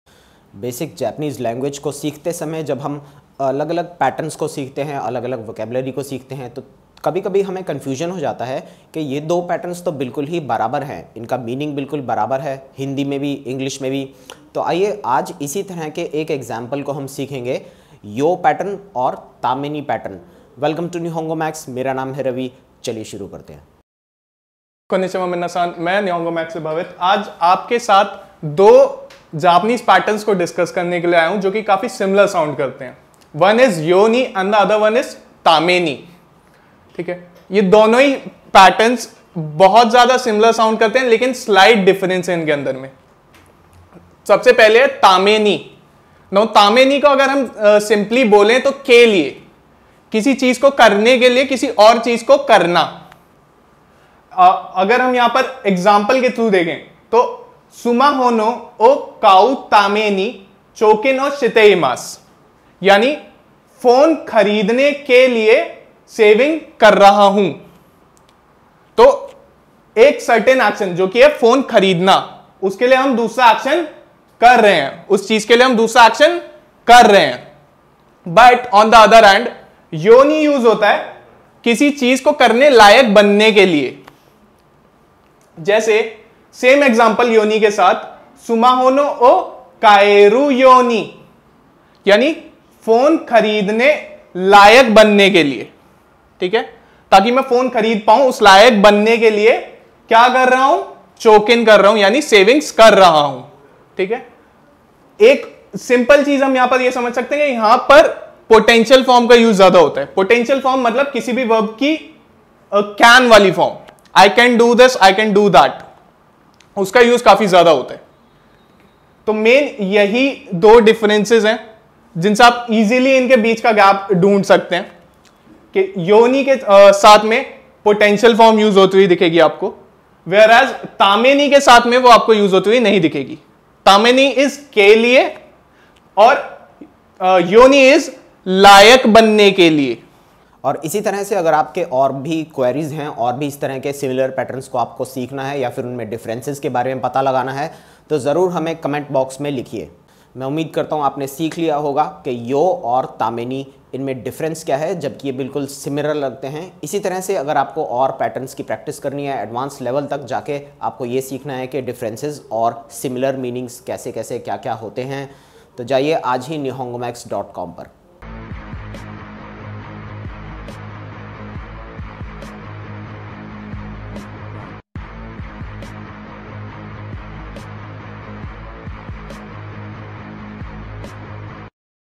Basic Japanese Difference of You and Tame
Hi All, To know about the Japanese language and to know about the real facts like how you can learn Japanese in a very short time